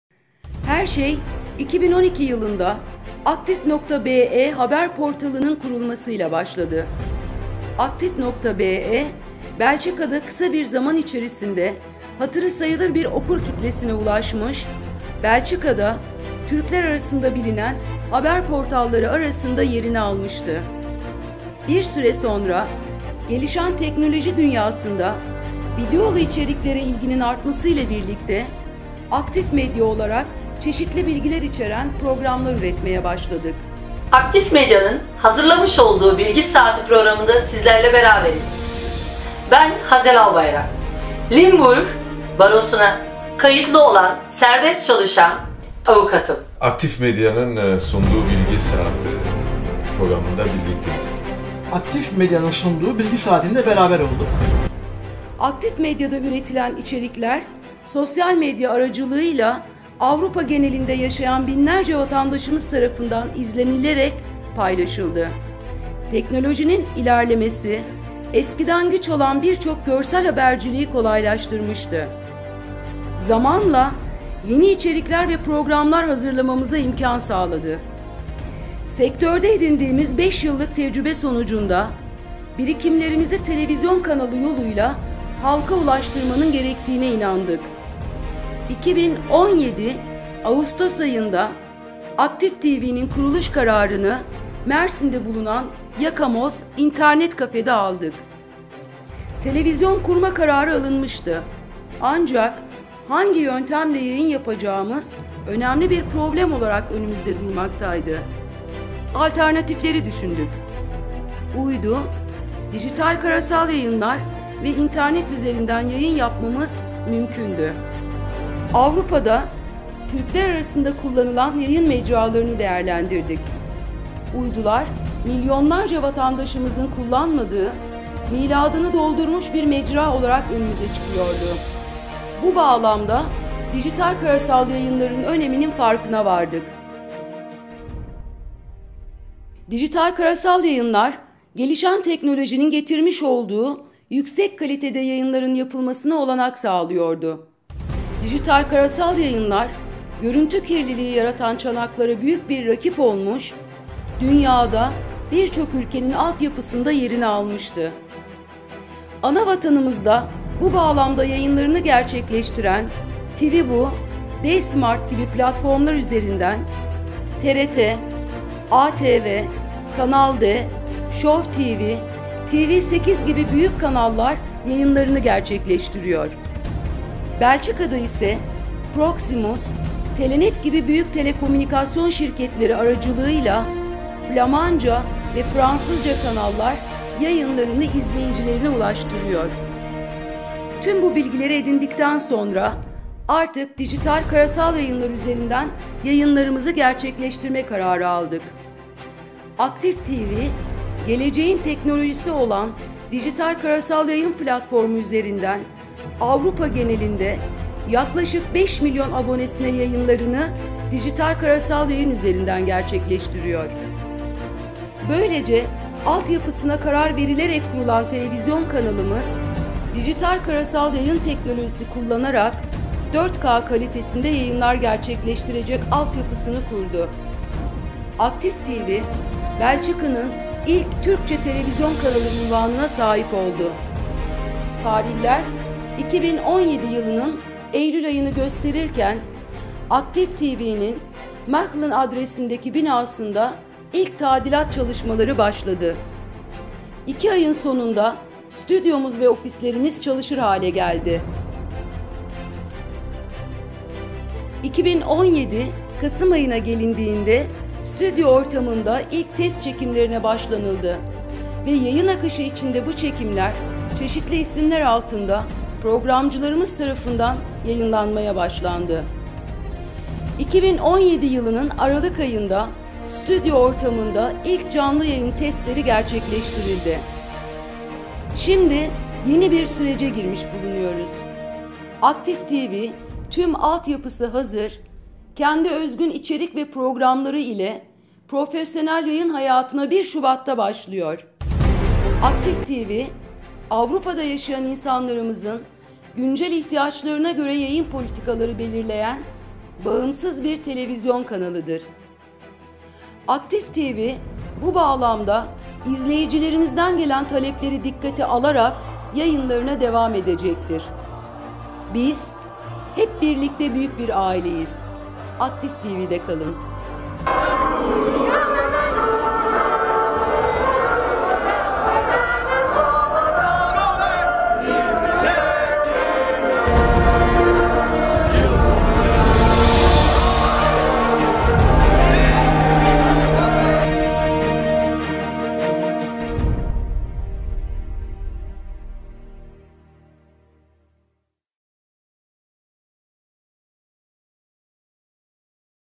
Aktif TV tanıtım belgeseli
Belçikalı Türklerin TV Kanalı Aktif TV​'nin tanıtım resepsiyonuna özel hazırlanan Belgeseli